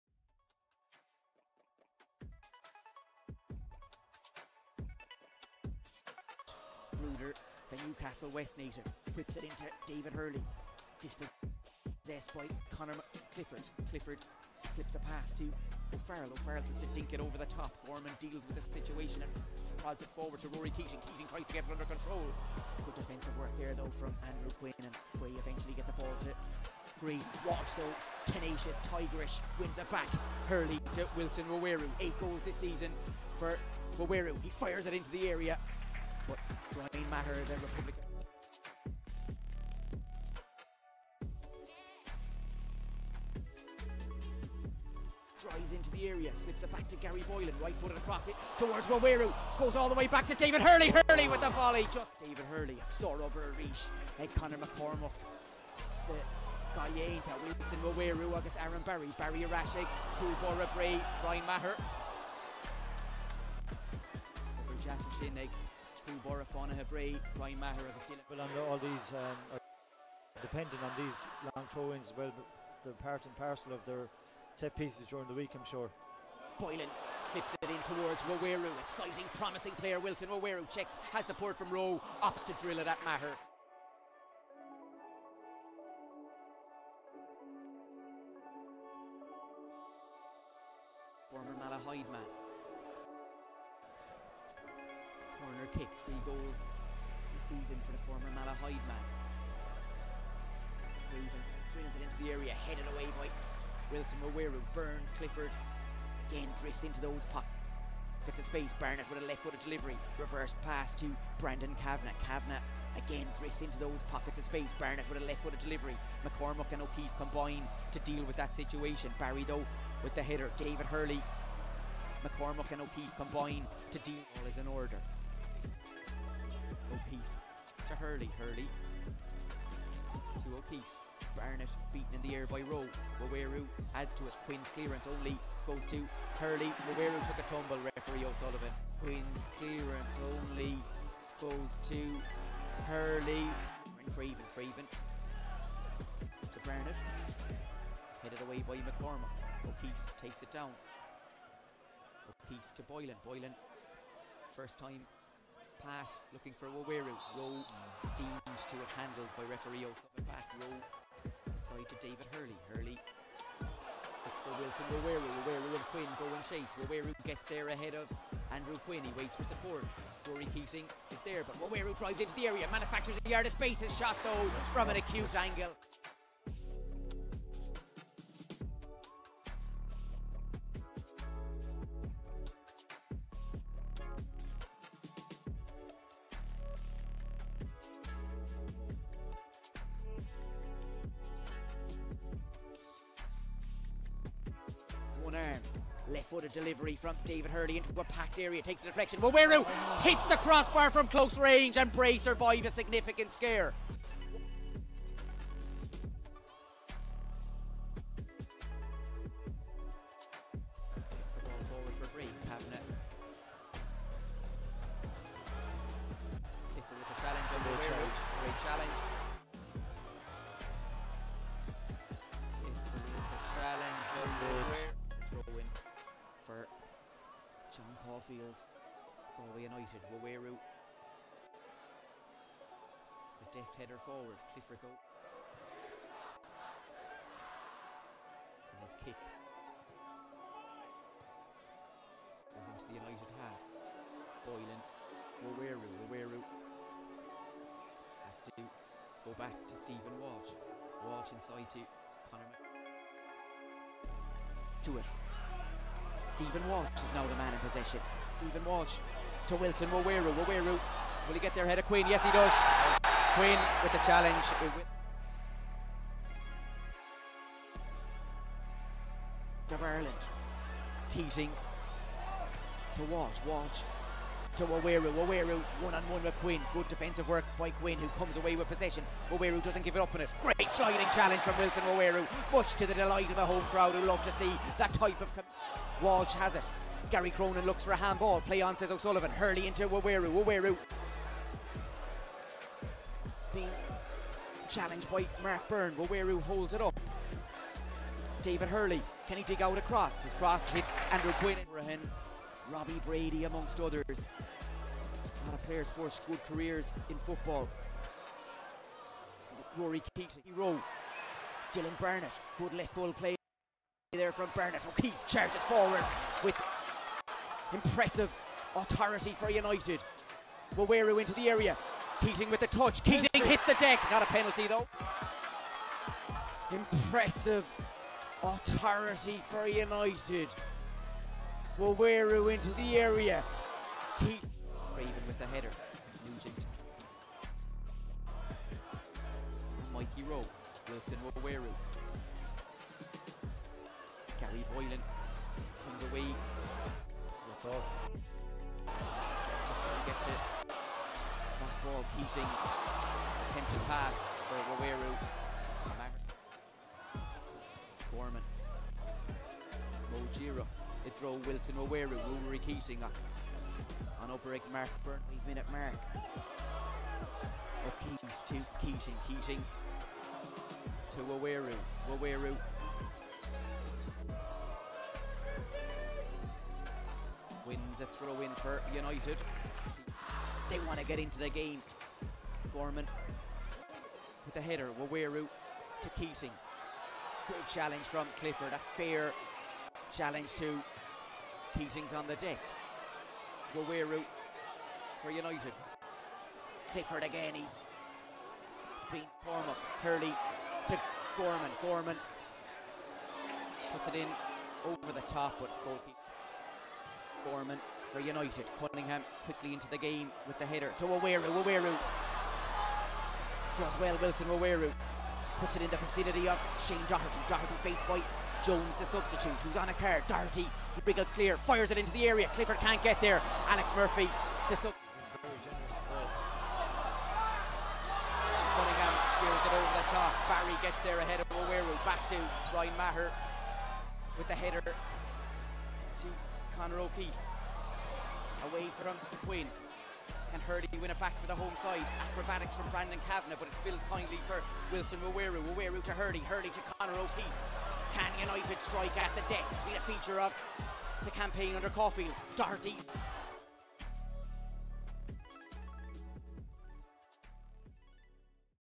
Wilson Waweru - 2nd leg Playoff Match Highlights - Galway United v Bray Wanderers - 07/11/2021
Strong match performance from Wilson Waweru for Galway United in their Promotion Playoff defeat to Bray Wanderers in the Ireland 1st Division

A constant attacking threat and a focal point of Galway United's attack. hitting the crossbar so close to equalising for his side

Unfortunate end to the season but a positive season from the talented young striker

24 games, 8 goals, 1 Assist & 1423 minutes played this season